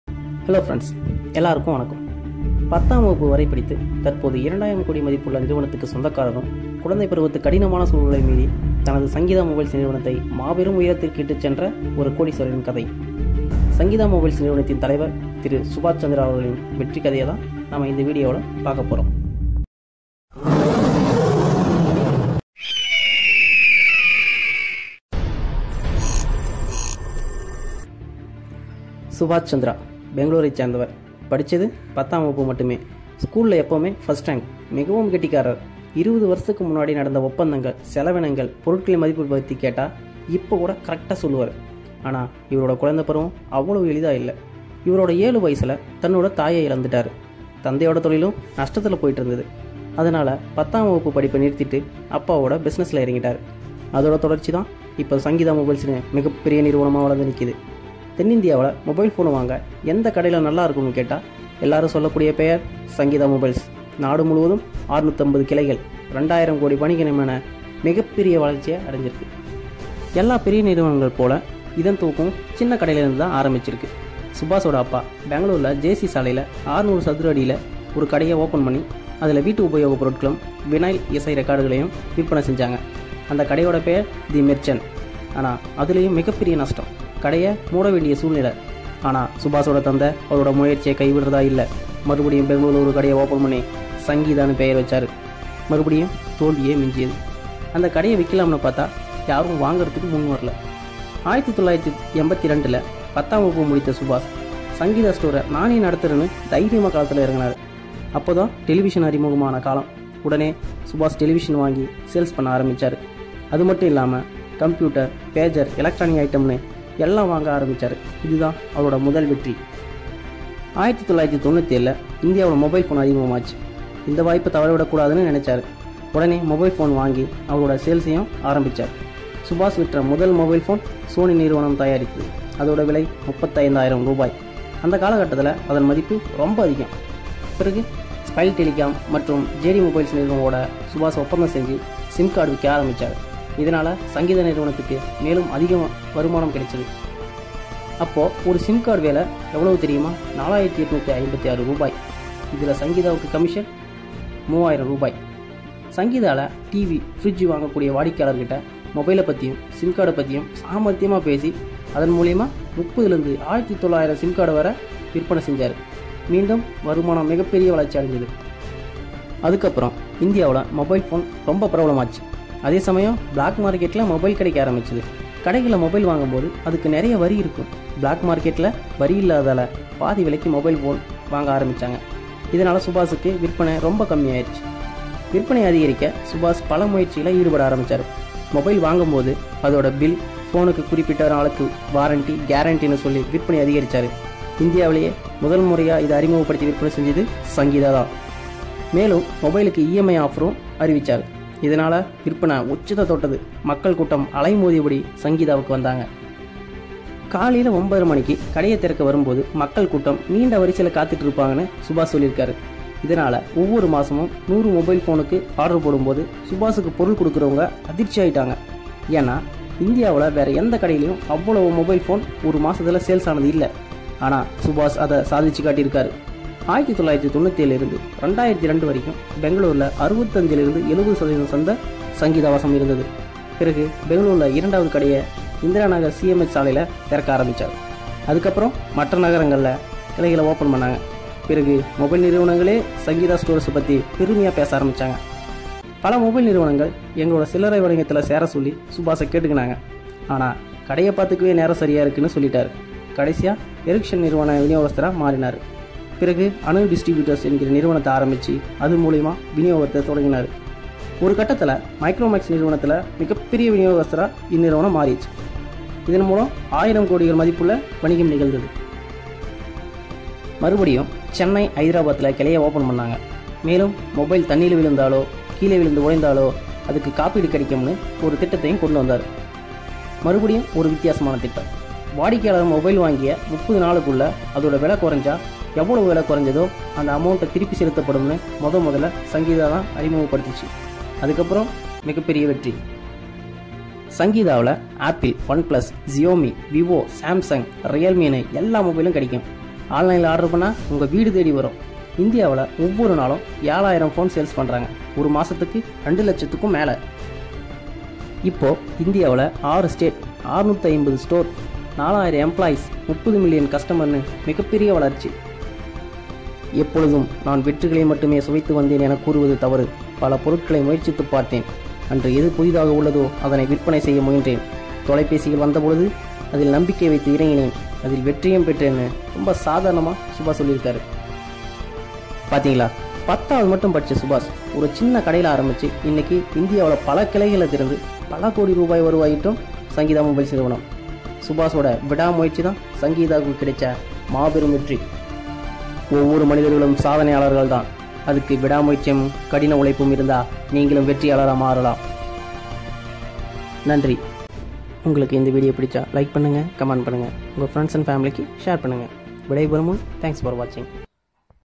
Sangeetha Mobiles - Success Story
பள்ளிப் படிப்பை பத்தாம் வகுப்போடு நிறுத்தி விட்டு இன்று Mobiles sales-ல் மிகப்பெரிய வளர்ச்சி கண்ட Sangeetha mobiles-ன் வெற்றி கதை.